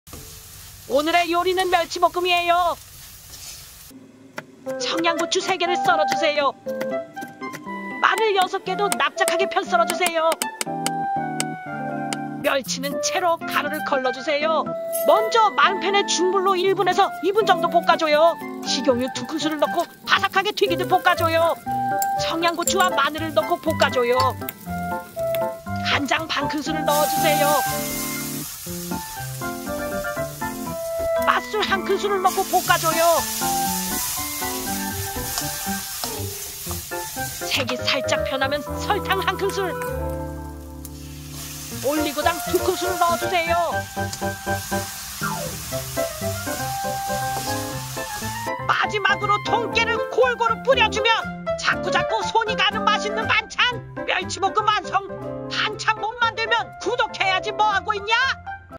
멸치 볶음 반찬 바삭 하고 맛있게 만드는 방법
밑반찬으로 인기있는 잔멸치 볶음 맛있게 만드는 방법이예요.
빠르게 뚝딱 만들어 밥이랑 같이 먹으면 정말 맛있어요.

[멸치볶음 준비물]
멸치 120g
설탕 1큰술
간장 1/2큰술
마늘 6알
통깨

[만드는 방법]

1. 청양고추와 마늘을 썰어주세요.
2. 멸치를 체반에 걸러주세요.
3. 중불로 마른팬에 멸치를 1~2분 볶아줘요
4. 식용유 2큰술 넣고 바삭하게 볶아줘요
5. 청양고추와 마늘을 넣고 함께 볶아줘요.
6. 진간장 1/2큰술을 넣어줘요..
7. 맛술 1큰술을 넣어줘요.
8. 설탕 1큰술을 넣어줘요.
9. 올리고당 2큰술을 넣고 마지막으로 볶아줘요.
10. 통깨를 뿌려주면 멸치볶음 완성!

멸치볶음 만드는방법
멸치볶음 만들기
멸치볶음 바삭하게
멸치볶음 황금 레시피

템플릿:론박자막연구소

이 콘텐츠는 인공지능 가상 연기자 서비스, 타입캐스트를 활용하여 제작되었습니다. 출연진: (숙희)